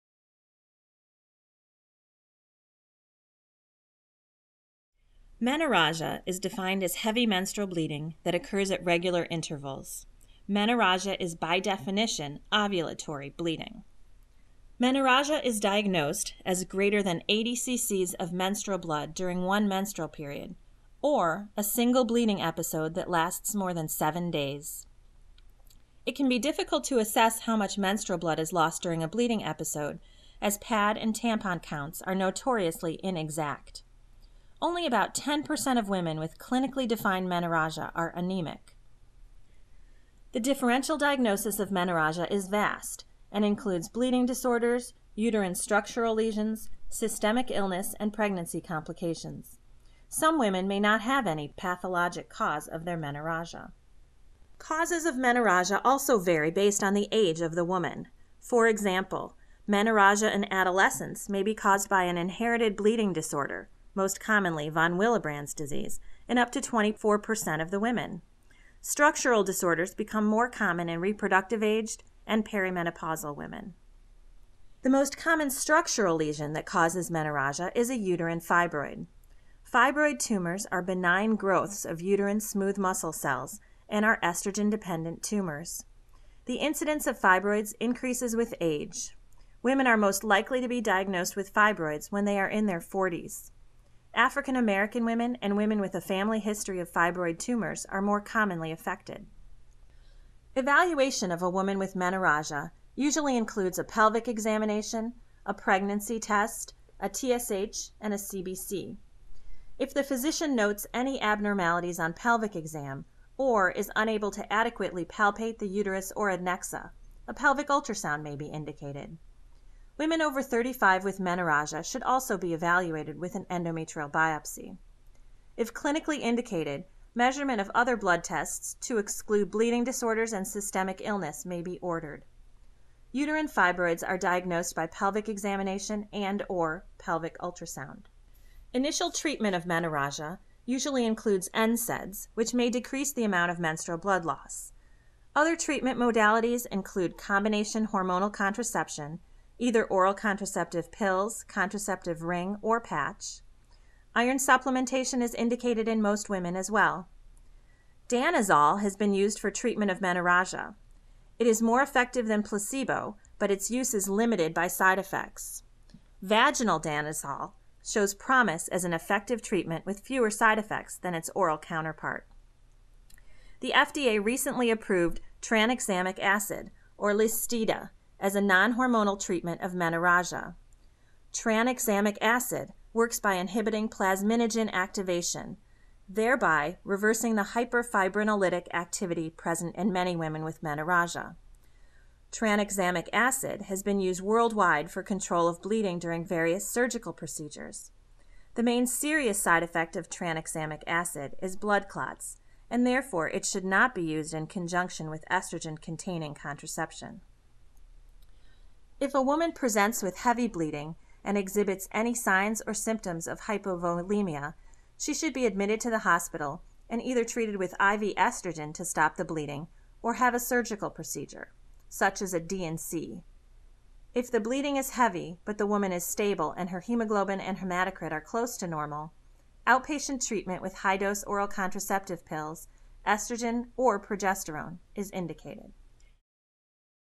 Menorrhagia
Dr. Sarina Schrager discusses menorrhagia in women.